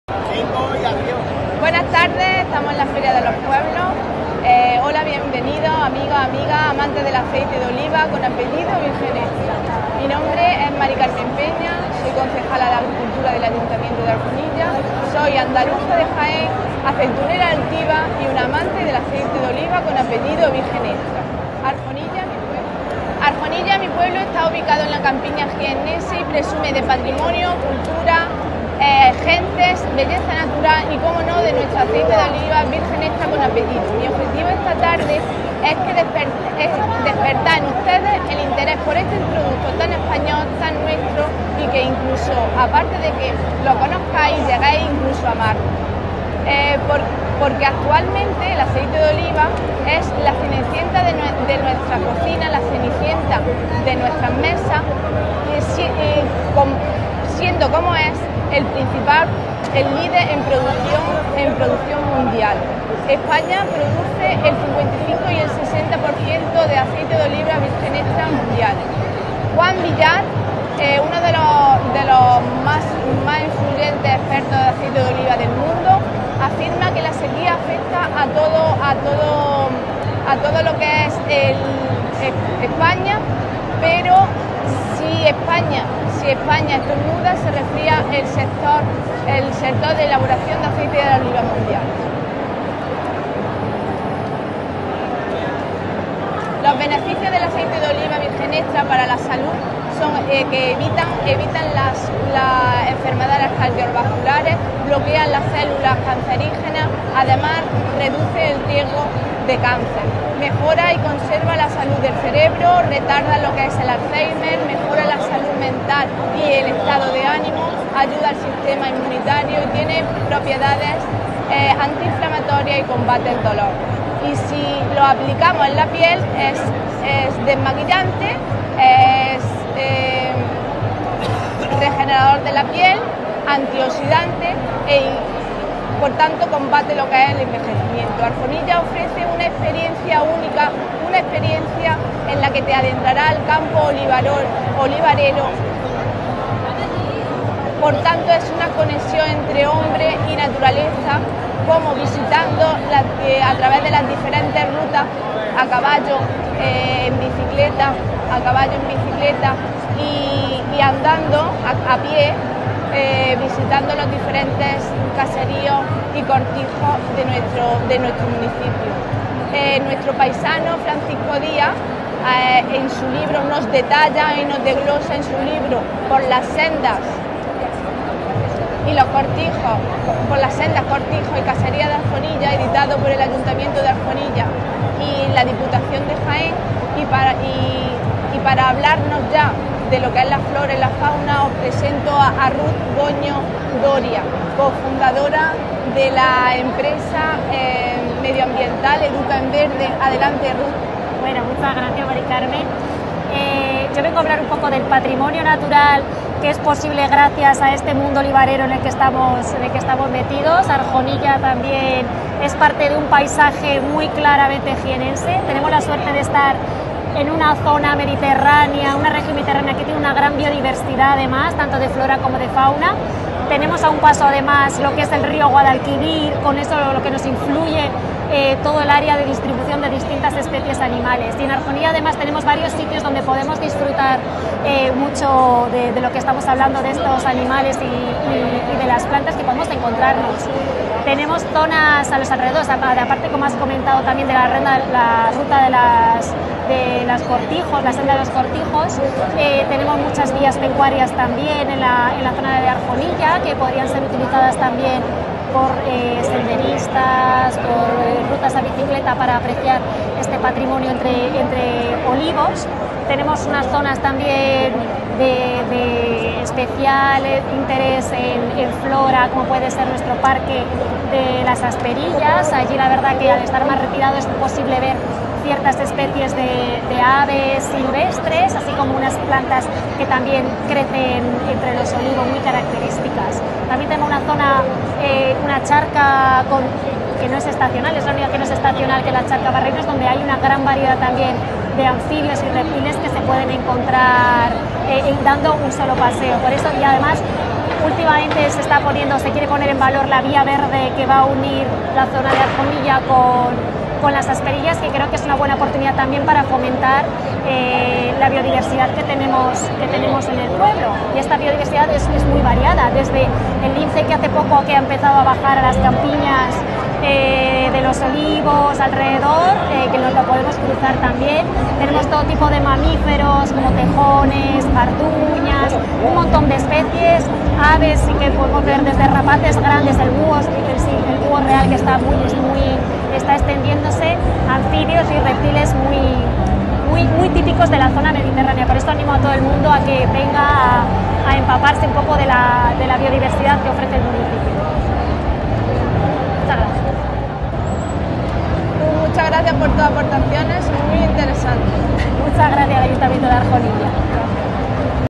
PATRMONIO NATURAL DE ARJONILLA OLIVOS, ACEITE, FLORA Y FAUNA LOCAL
Feria de los Pueblos 2024.
Interviene.
- Dª. Maria del Carmen Peña Cañones.
(Concejala de Agricultura y Educación del Ayto de Arjonilla).
- Dª. Ruth Goñi.
(Cofundadora de Educa en Verde).